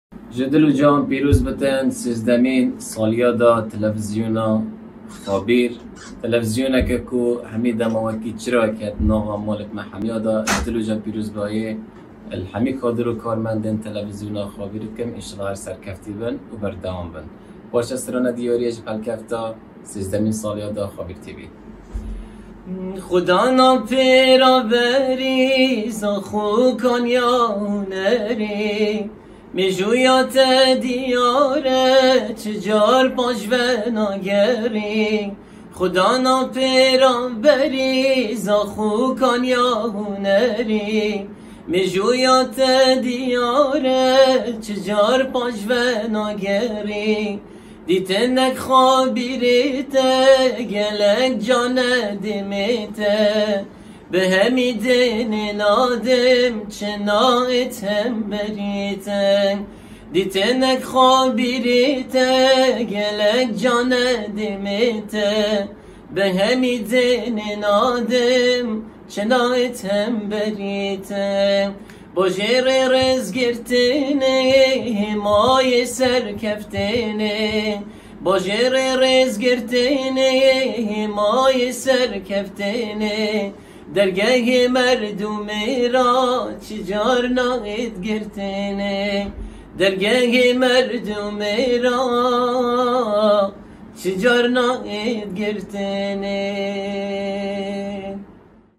پیرۆزباهیێن هونەرمەند ( جادر زاخوى ) ب هەلکەفتا 13 مین سالڤەگەرا تەلەفزیونا خابیر
ـــ
هەمى ئەکوانتێن دەزگەهێ خابیر